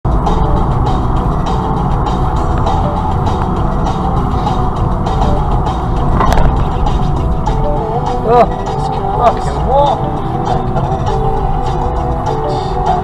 Stupid driver
The dickhead in the silver car doesn't even bother stopping at the line, just pulls out in front of the blue mpv.